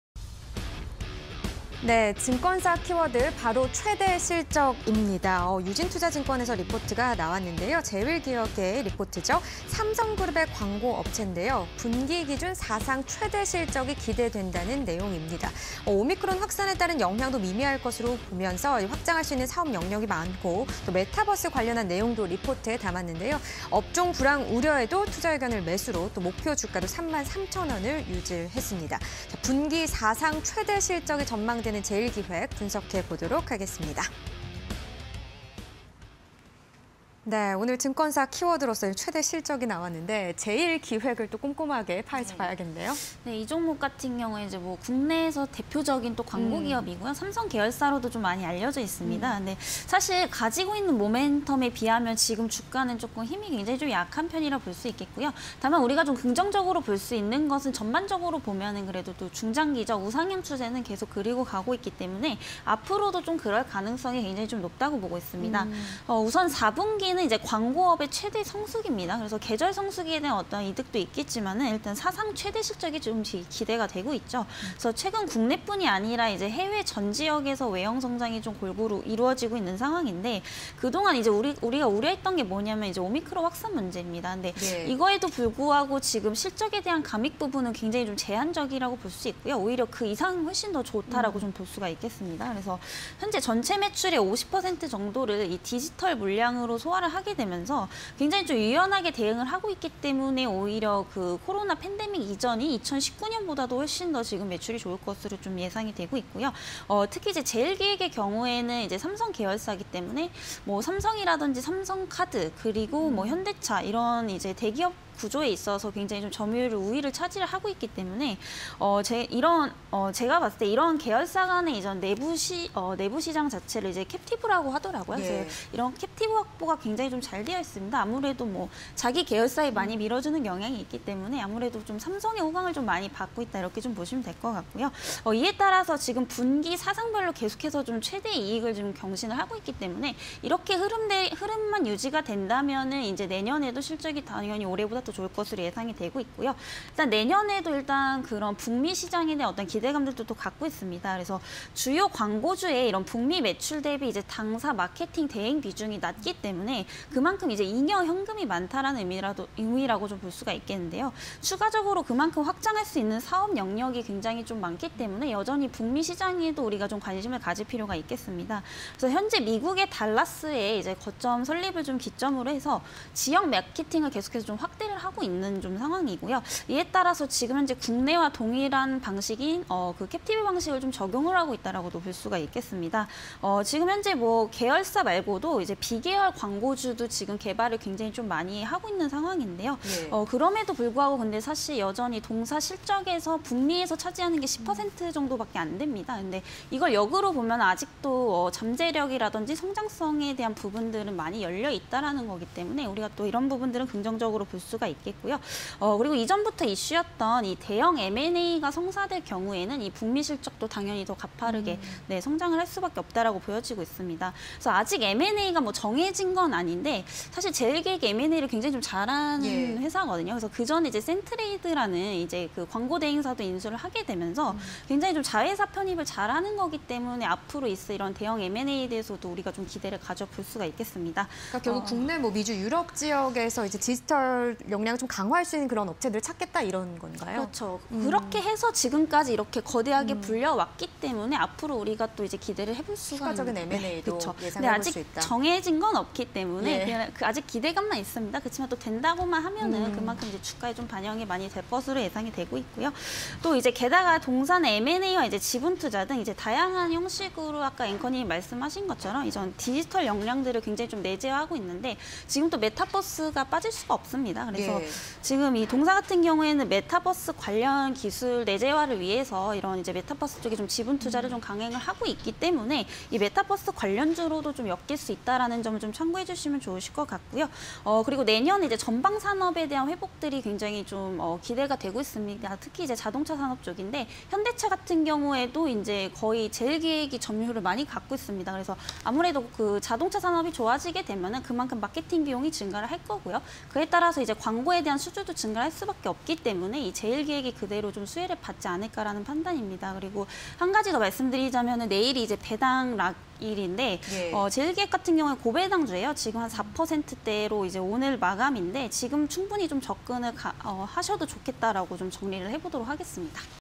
제일기획(030000)... 4분기 사상 최대 실적 전망_키워드 팩토리 (20211228)
제일기획(030000)... 최대 실적
- 4분기 매출 3,510억(+14.9%) 영업이익 710억(+7.8%) 전망
- 분기 사상 최대 실적 전망
- 디지털 물량으로 전체 매출 50% 소화
- 삼성 계열사로 광고 수주 점유율↑
- 북미 시장 성장 기대감 확대
- 대형 M&A 성사 시 가파른 성장 예상
- 올해 고배당 수익 기대